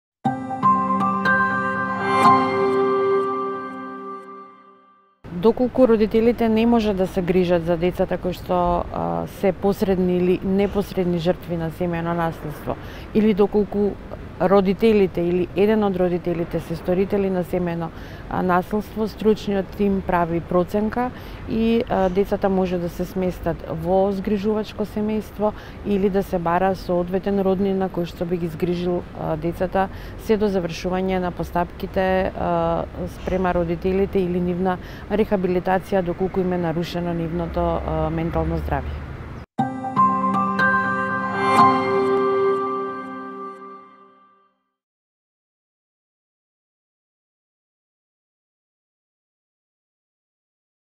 Што се случува со децата кога родителот не може да се грижи за нив поради насилство?
Одговара Наташа Станојевиќ од Центарот за социјална работа – Скопје.